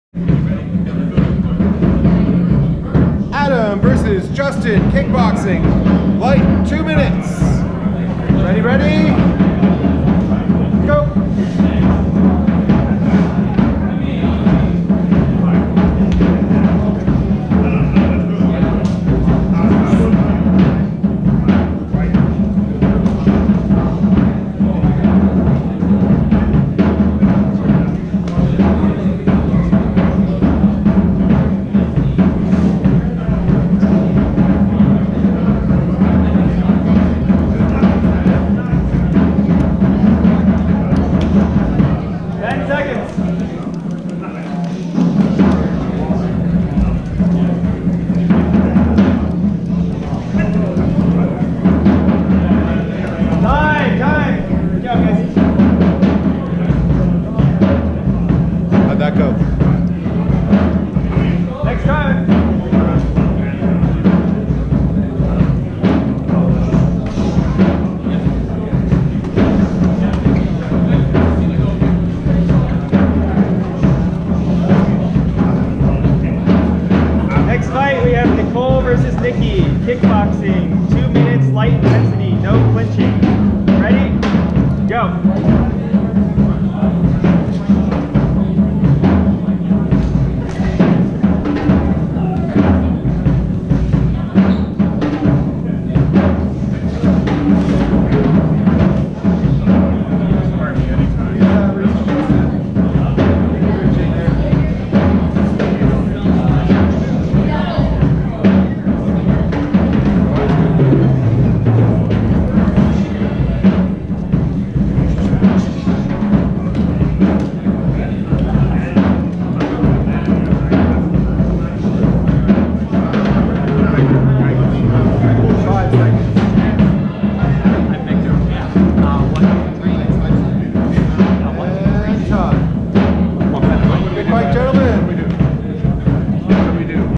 oronto Throwdown Group - This event was run in Oct 2019. All the fights as part of this listing are meant for personal review by the fighters. If your comments on any of these videos are anything less than constructive they will be removed.